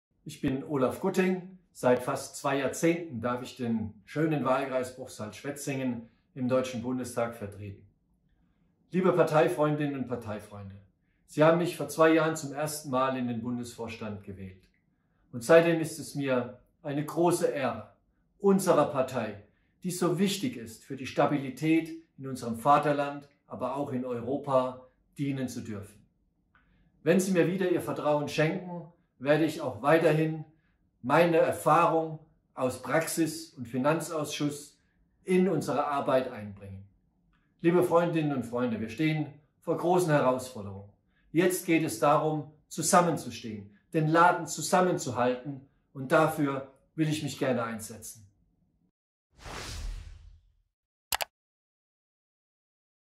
Kandidatur Olav Gutting MdB
Kandidat für den Bundesvorstand - vorgeschlagen von: CDU Baden-Württemberg